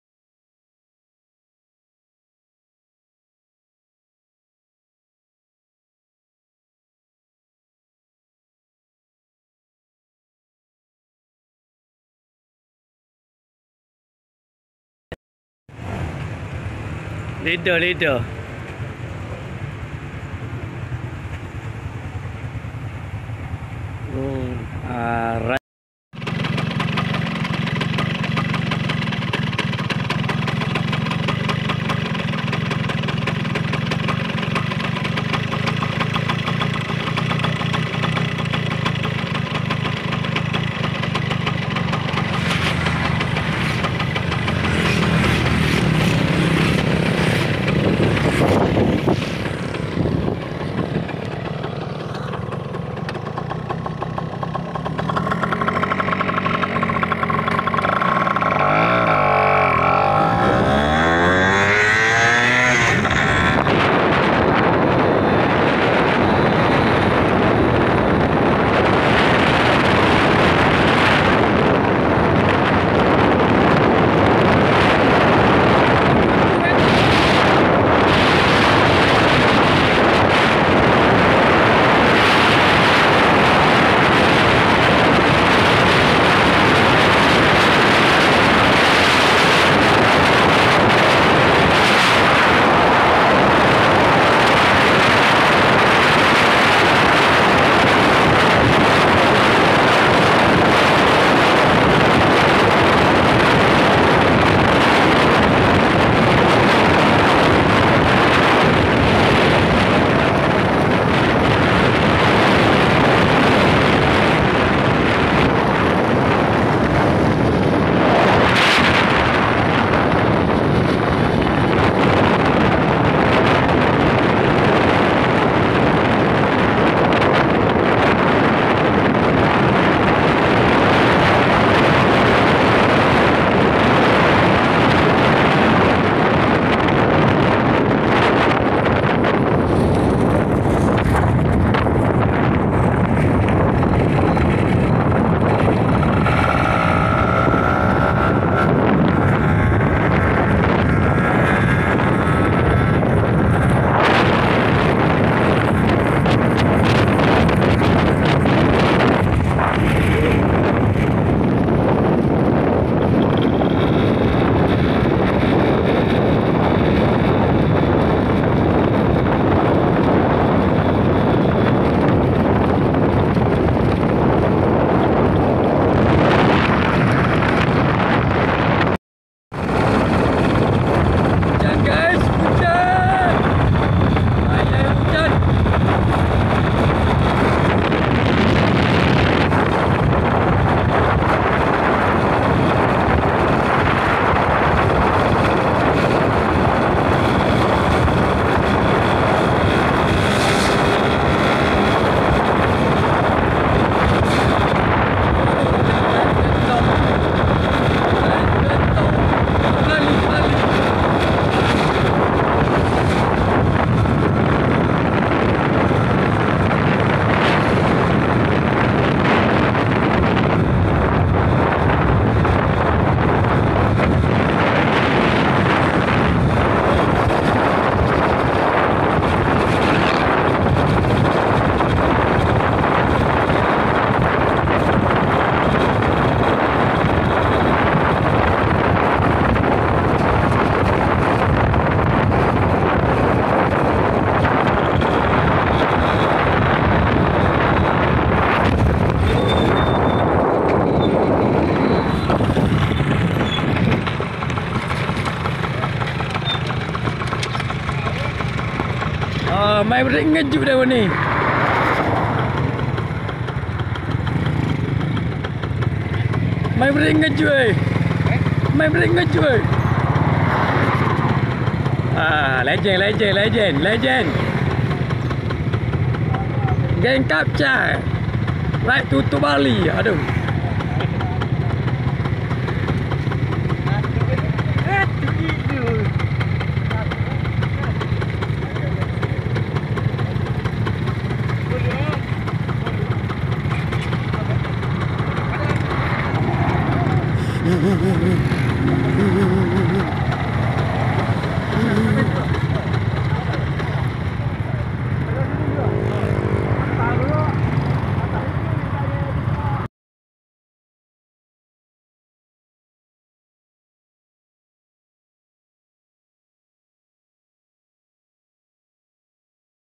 Test moto aku yamaha ss110 jalan jauh sikit..
Ride suka2 mencari pulut ikan bakar..